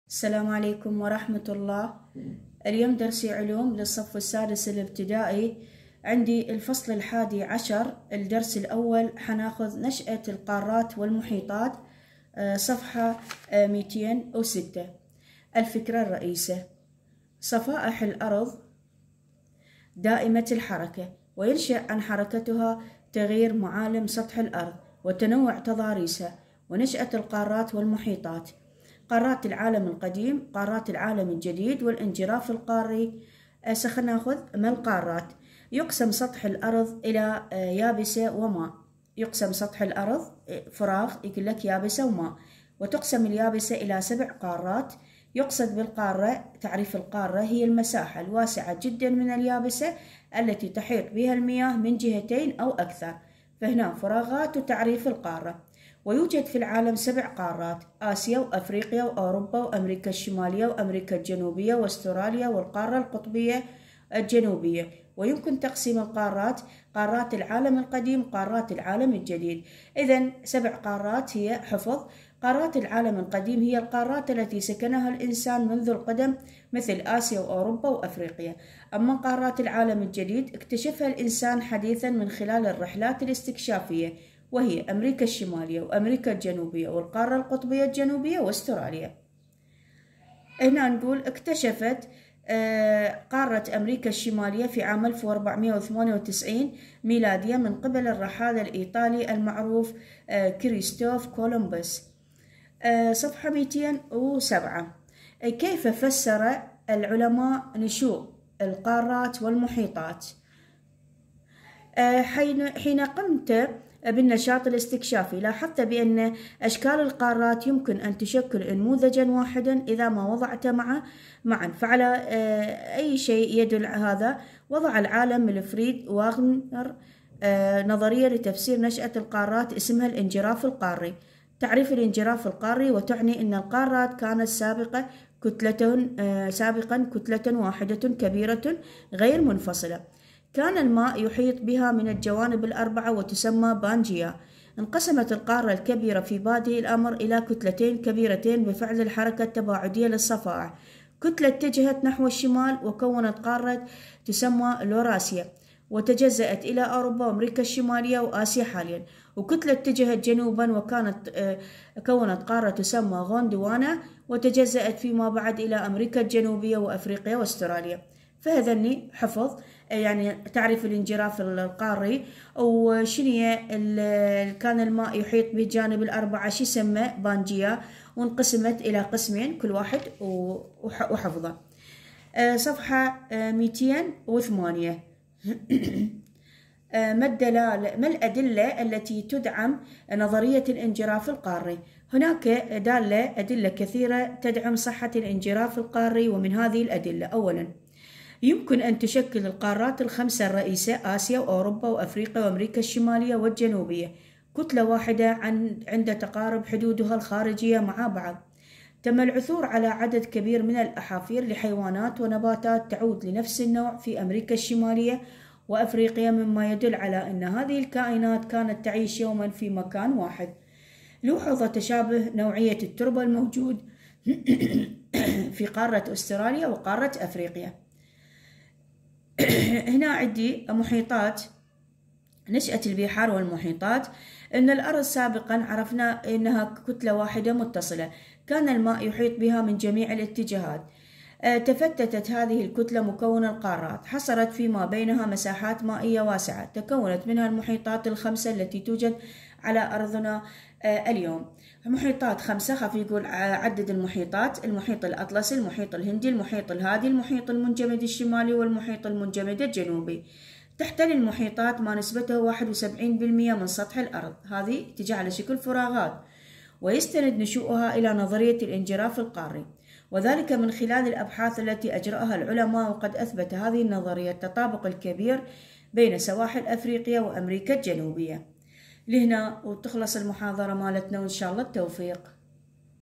السادس الابتدائي الفصل ال ١١ ألدرس الاول( صفحة ٢٠٦) نشاة القارات والمحيطات
علوم للصف السادس الابتدائي الفصل ال ١١
الدرس الأول صفحة ٢٠٦
وقناتي ع التليكرام الجديدة 👇
لجميع الأسئلة والحلول والاخبار العامة